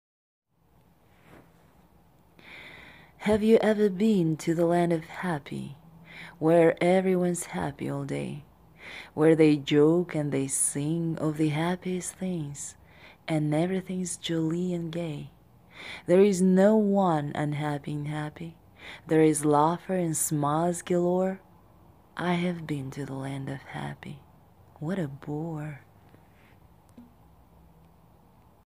The Land Of Happy - by Shel Silverstein
narrator Reading Fox
illustrated by L'Ours qui dessine

Have you been to the land of happy,
Where everyone's happy all day,
Where they joke and they sing
Of the happiest things,
And everything's jolly and gay?
There's no one unhappy in Happy
There's laughter and smiles galore.
I have been to The Land of Happy-
What a bore